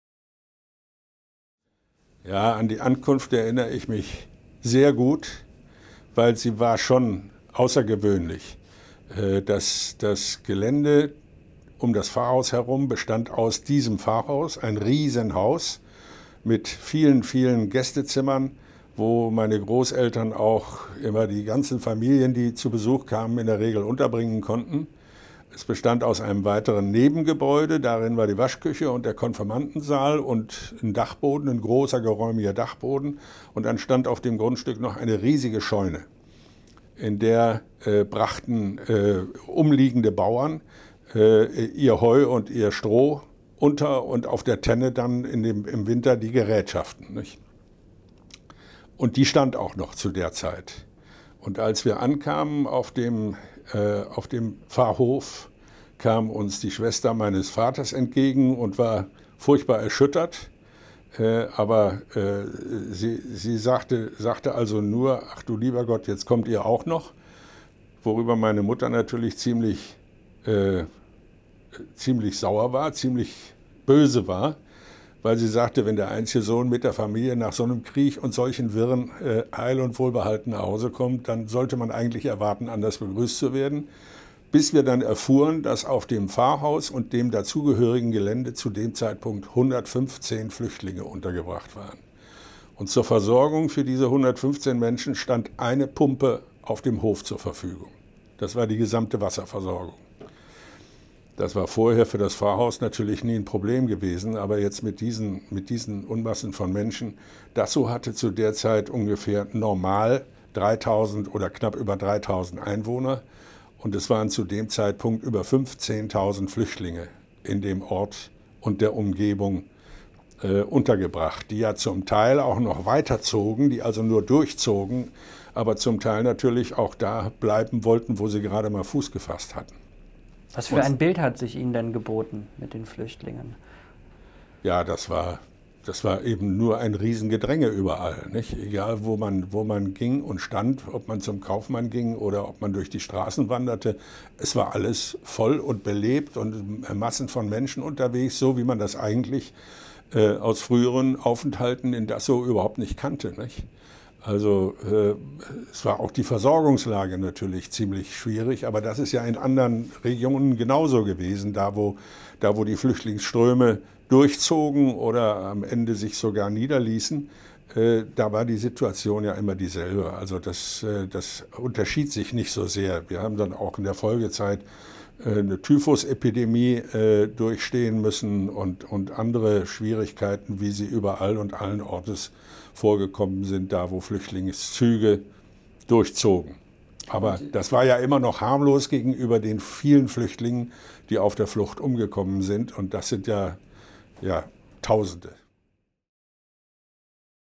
Klaus Romberg: Überrant von Flüchtlingsmassen
Klaus Romberg war gegen Ende des Zweiten Weltkriegs mit seiner Familie von Leipzig nach Dassow in Mecklenburg geflohen - die meiste Strecke auf dem Fahrrad. Die Schwester seines Vaters war alles andere als erfreut, als sie ihre Verwandten auf den Hof radeln sah: Sie beherbergte bereits über 100 Flüchtlinge aus dem Osten.